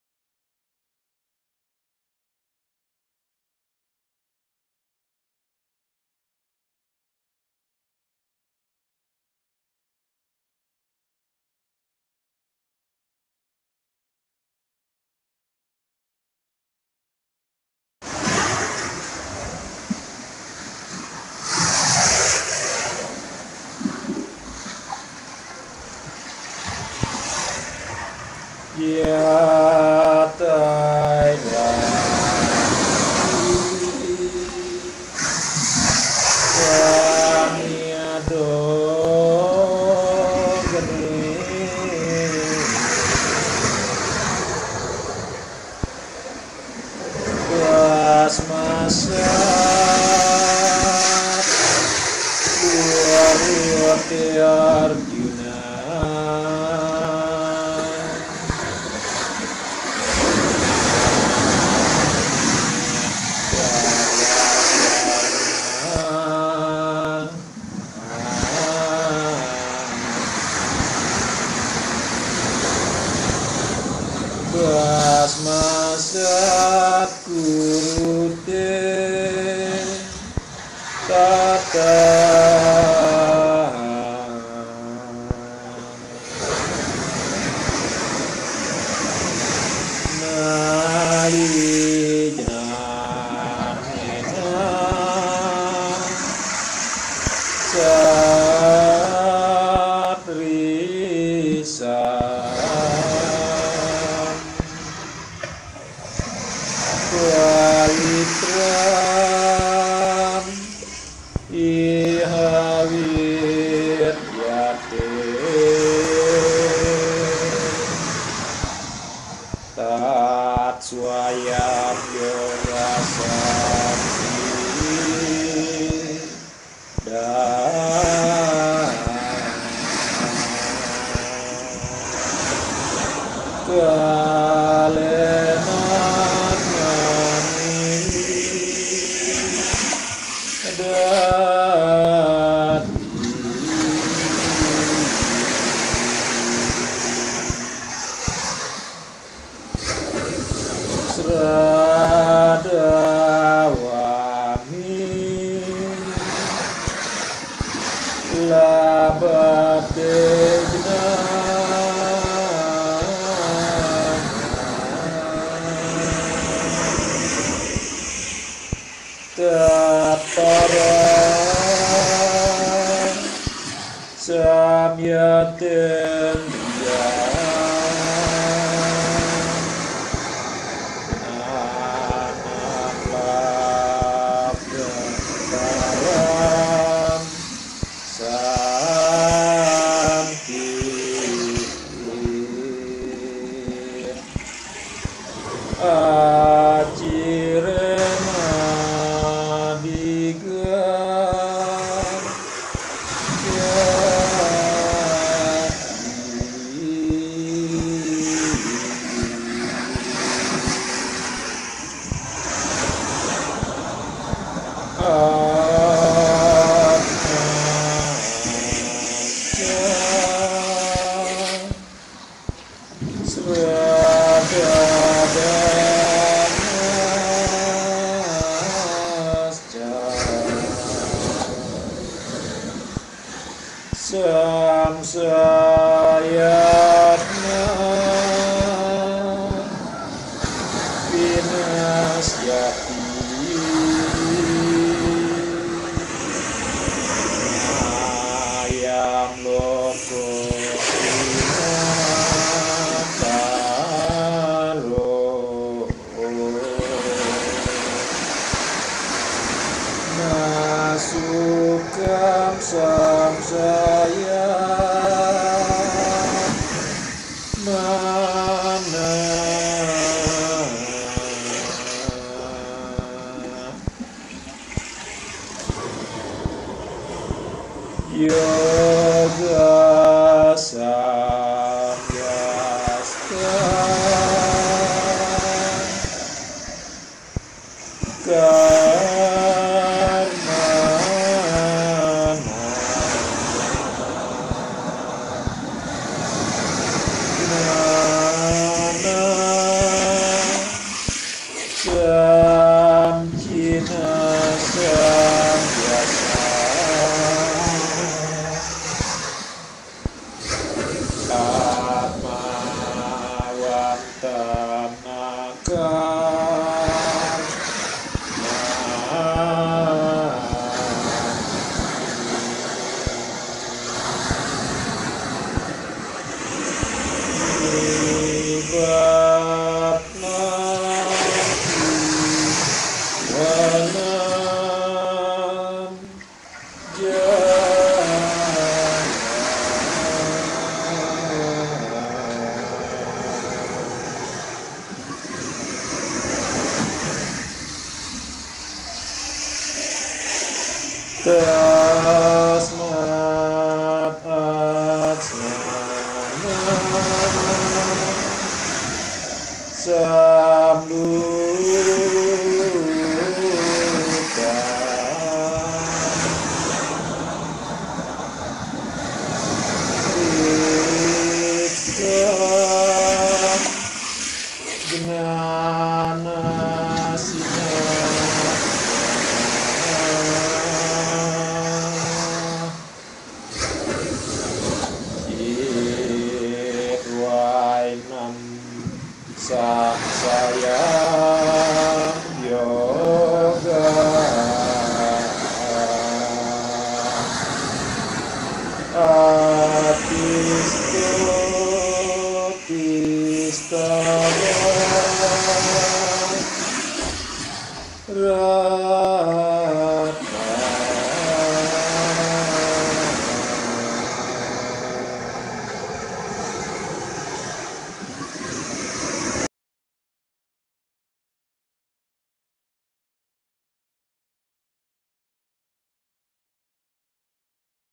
Bhagavadgita, Percakapan Keempat_5: Indonesian and English Subtittle
... setelah memotong keraguan dalam hatimu karena ketidaktahuanmu, dengan ilmu pengetahuan tentang diriKU berpegang pada yoga, Bangkit dan Berperanglah oh Bharata (Krishna)